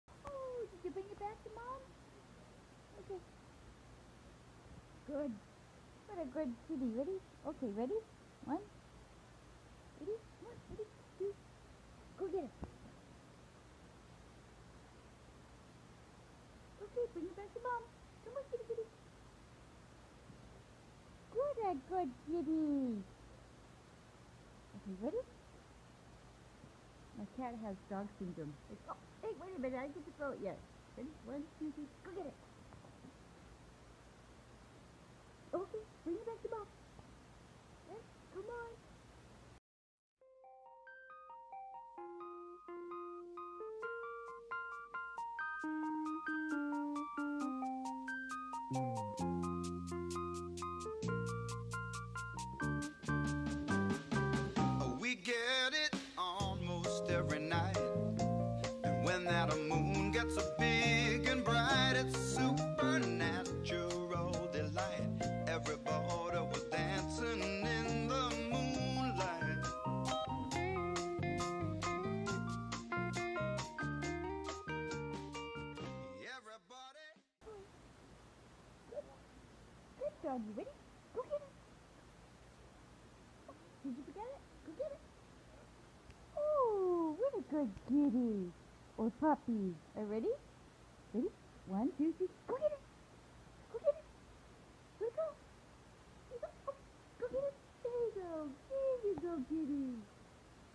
My kitten thinks it is a dog.. It fetches
Kitten fetches like a dog..